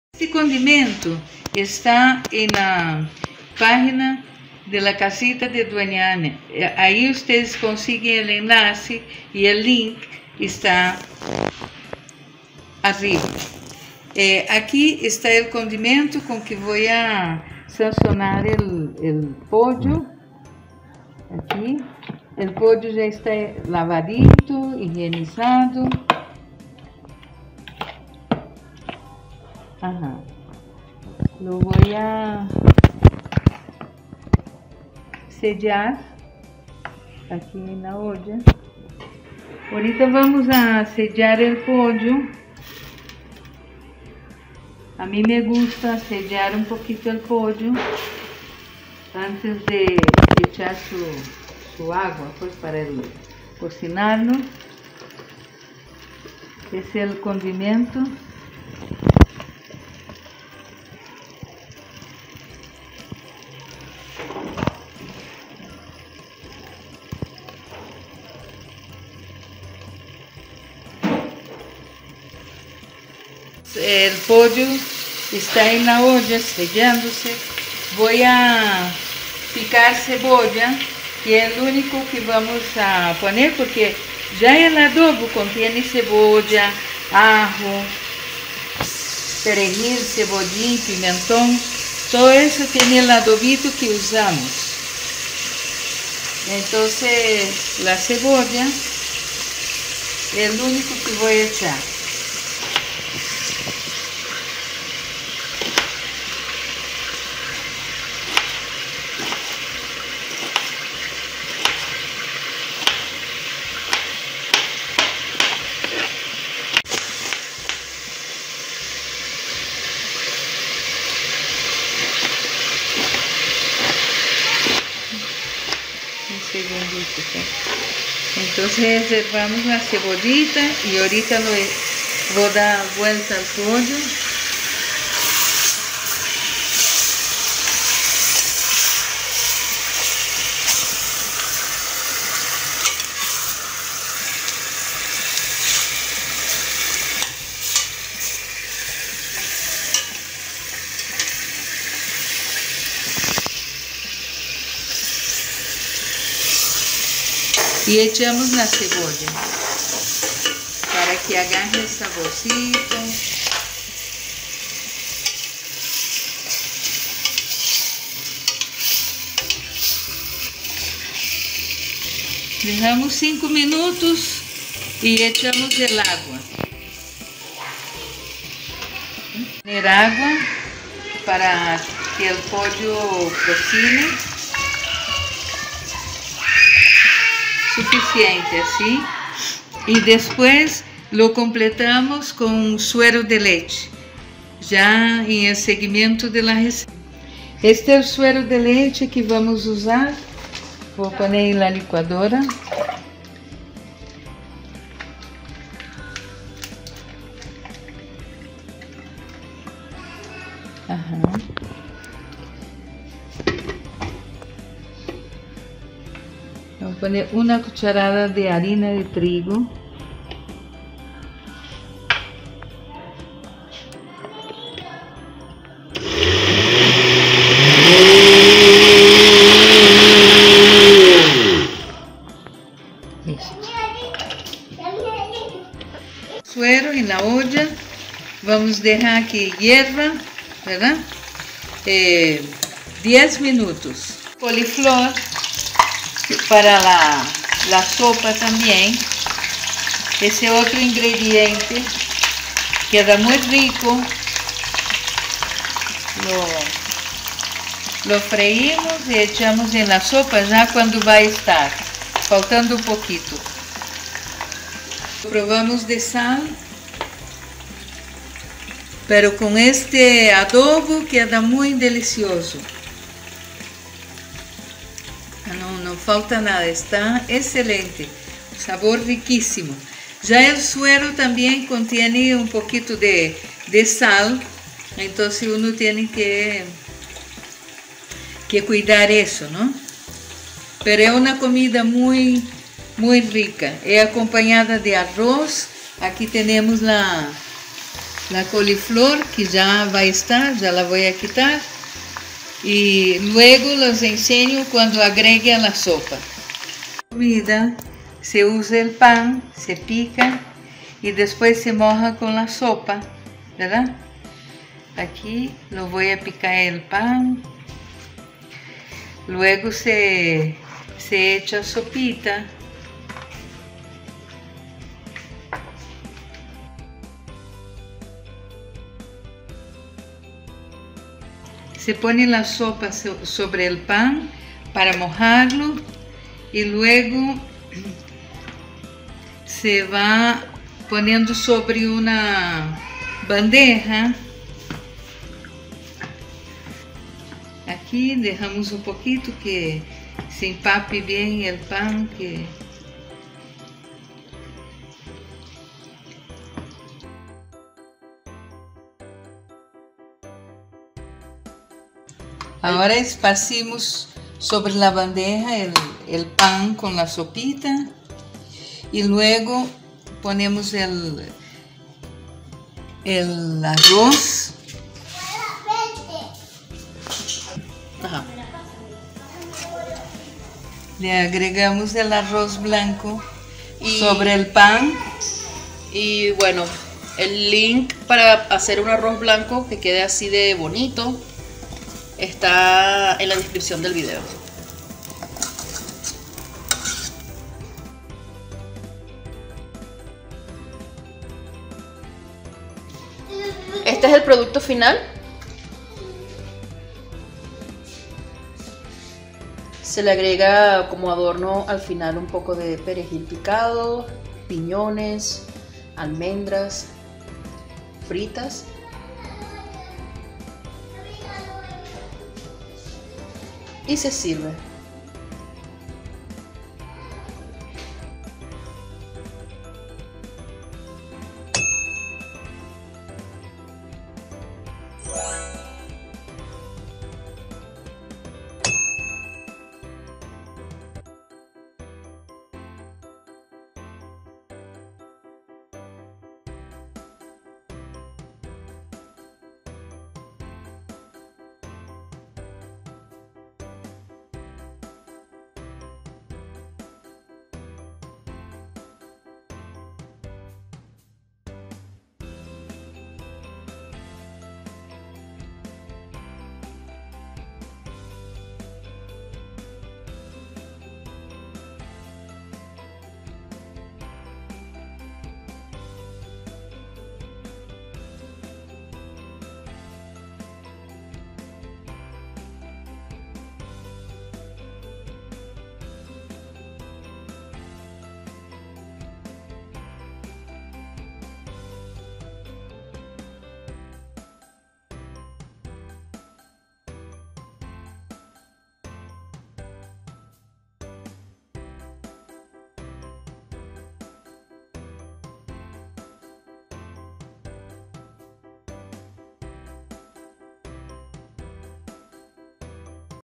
Mi Version del Mansaf Sopa de Suero de Leche con Arroz y Pollo
En el video de hoy te mostraré   mi Versión del Mansaf Sopa de Suero de Leche con Arroz y Pollo

Clic aquí en el siguiente enlace para acceder a las mejores recetas y oportunidades de emprender tu propio negocio de comida 👇

El Mansaf es un plato árabe tradicional hecho originalmente con cordero cocinado en una sopa de yogur seco fermentado conocido como(Qishek) y servido con arroz blanco y pan árabe. Pero en nuestra versión conocerás el Mensaf con pollo o arroz con pollo y suero de leche.

El mansaf es  un platillo popular en muchos países árabes como Palestina, Irak, Israel, el Sur de Siria y Arabia Saudita. Es el plato nacional de Jordania y se suele comer con la mano  en una misma bandeja utilizando solo los dedos índice, el del medio y el pulgar .

En este canal encontraras todo tipo de comida arabe recetas en español.